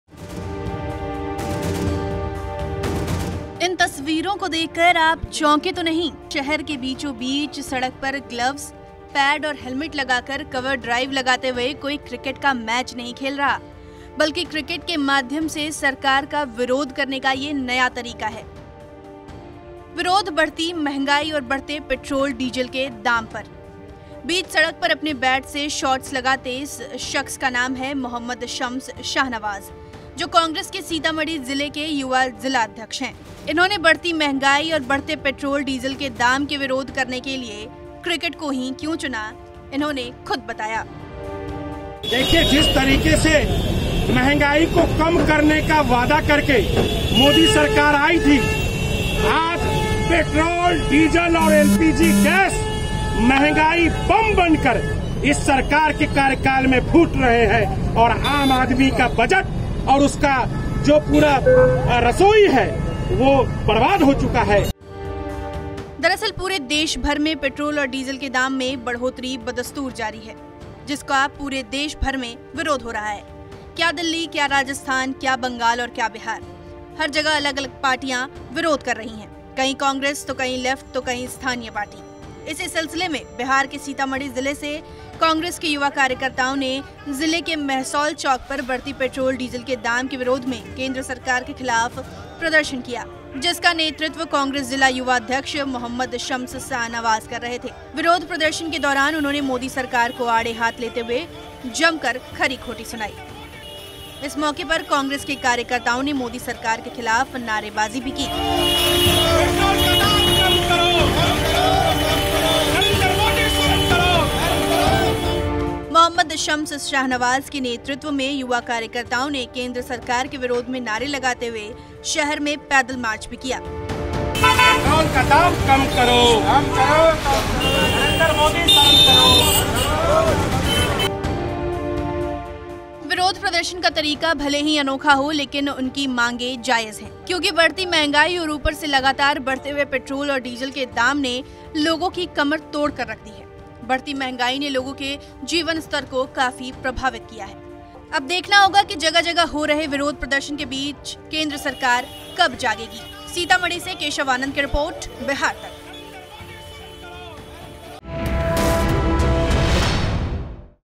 Petrol-Diesel के बढ़ते दामों पर ये अनोखा प्रदर्शन देखा क्या । Bihar Tak
इन तस्वीरों को देखकर आप चौंके तो नहीं. एक जो शहर के बीचों बीच सड़क पर  गलव्स ,पैड और हेलमेट लगाकर कवर ड्राइव लगाते हुए कोई क्रिकेट का मैच नहीं खेल रहा है. बल्कि क्रिकेट के माध्यम से सरकार का विरोध करने का नया तरीका निकाला है. विरोध बढ़ती मंहगाई और बढ़ते पेट्राल, डीजल के दाम का..